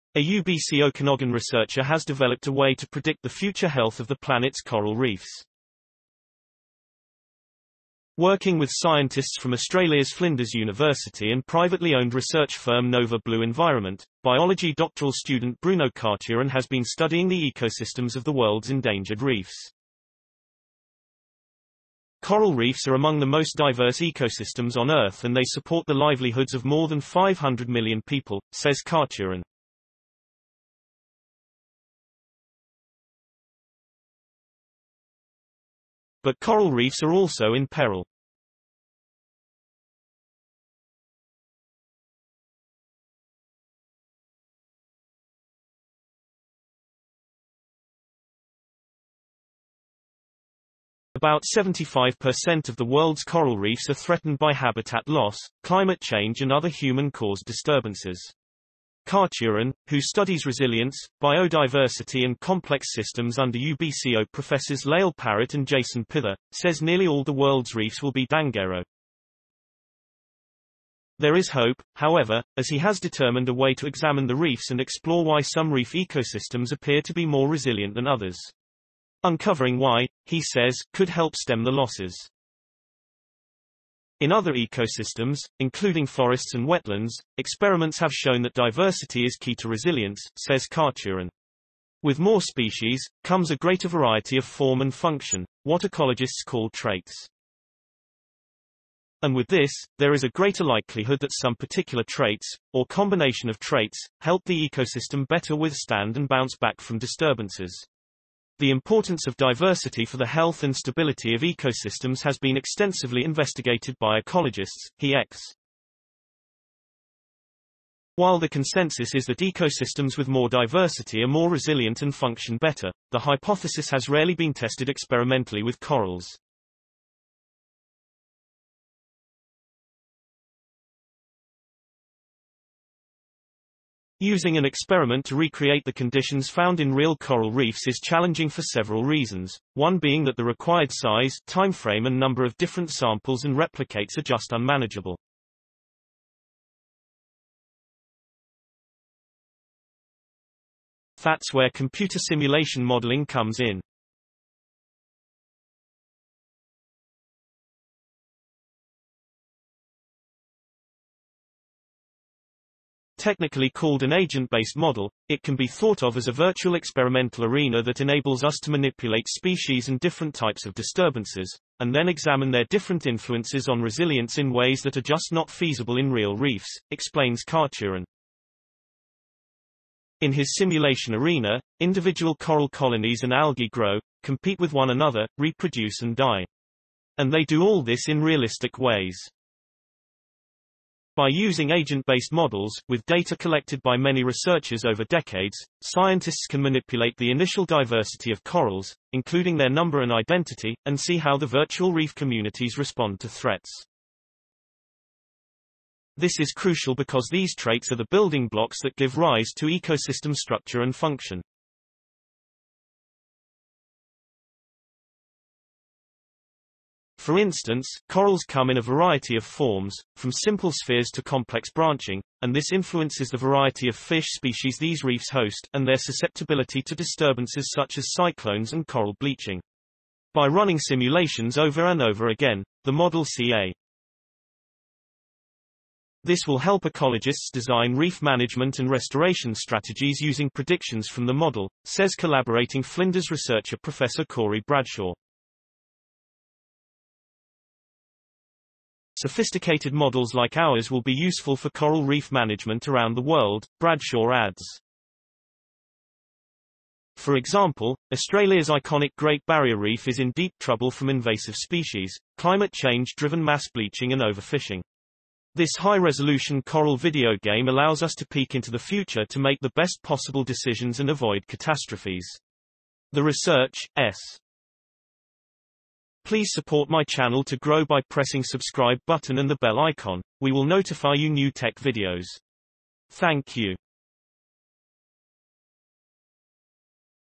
Computer modeling used to predict reef health
Computer modeling used to predict reef health - Information for all latest updates Science and Technology

A UBC Okanagan researcher has developed a way to predict the future health of the planet's coral reefs. Working with scientists from Australia's Flinders' University and privately-owned research firm Nova Blue Environment, biology doctoral student Bruno Carturan has been studying the ecosystems of the world's endangered reefs.
"Coral reefs are among the most diverse ecosystems on Earth and they support the livelihoods of more than 500 million people," says Carturan. "But coral reefs are also in peril. About 75 per cent of the world's coral reefs are threatened by habitat loss, climate change and other human-caused disturbances."
Carturan, who studies resilience, biodiversity and complex systems under UBCO Professors Lael Parrott and Jason Pither, says nearly all the world's reefs will be dangerously affected by 2050 if no effective measures are taken.
There is hope, however, as he has determined a way to examine the reefs and explore why some reef ecosystems appear to be more resilient than others. Uncovering why, he says, could help stem the losses.
"In other ecosystems, including forests and wetlands, experiments have shown that diversity is key to resilience," says Carturan. "With more species, comes a greater variety of form and function -- what ecologists call traits. And with this, there is a greater likelihood that some particular traits, or combination of traits, help the ecosystem better withstand and bounce back from disturbances."
The importance of diversity for the health and stability of ecosystems has been extensively investigated by ecologists, he explains. While the consensus is that ecosystems with more diversity are more resilient and function better, the hypothesis has rarely been tested experimentally with corals. Using an experiment to recreate the conditions found in real coral reefs is challenging for several reasons -- one being that the required size, timeframe and number of different samples and replicates are just unmanageable.
That's where computer simulation modelling comes in.
"Technically called an 'agent-based model', it can be thought of as a virtual experimental arena that enables us to manipulate species and different types of disturbances, and then examine their different influences on resilience in ways that are just not feasible in real reefs," explains Carturan.
In his simulation arena, individual coral colonies and algae grow, compete with one another, reproduce and die. And they do all this in realistic ways. By using agent-based models -- with data collected by many researchers over decades -- scientists can manipulate the initial diversity of corals, including their number and identity, and see how the virtual reef communities respond to threats.
"This is crucial because these traits are the building blocks that give rise to ecosystem structure and function. For instance, corals come in a variety of forms -- from simple spheres to complex branching -- and this influences the variety of fish species these reefs host, and their susceptibility to disturbances such as cyclones and coral bleaching."
By running simulations over and over again, the model can identify combinations that can provide the greatest resilience. This will help ecologists design reef management and restoration strategies using predictions from the model, says collaborating Flinders researcher Professor Corey Bradshaw. "Sophisticated models like ours will be useful for coral-reef management around the world," Bradshaw adds. "For example, Australia's iconic Great Barrier Reef is in deep trouble from invasive species, climate change-driven mass bleaching and overfishing."
"This high-resolution coral 'video game' allows us to peek into the future to make the best possible decisions and avoid catastrophes."
The research, supported by grants from the Natural Sciences and Engineering Research Council of Canada and the Canada Foundation for Innovation, was published recently in eLife.